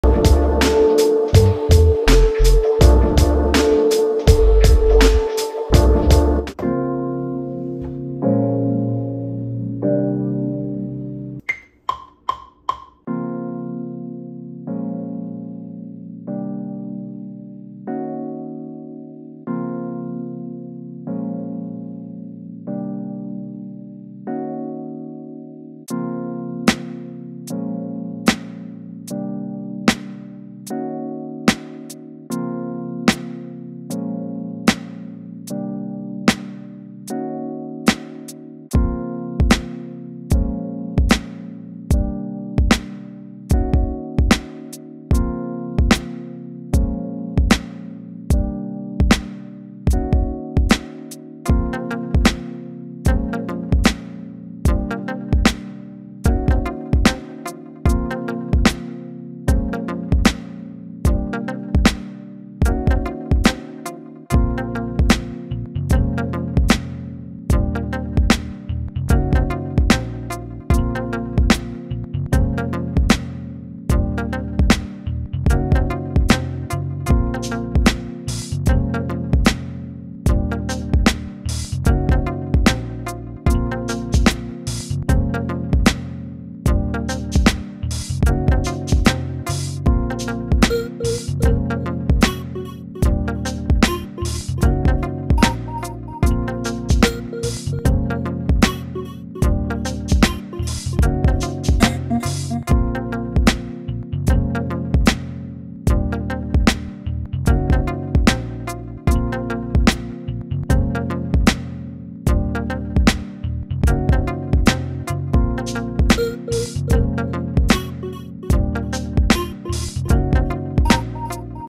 Beatmaking On Maschine Mikro With The Velvet Lounge Expansion
Making a chill hip hop beat with sounds from the Velvet Lounge Expansion from Native Instruments on Maschine Mikro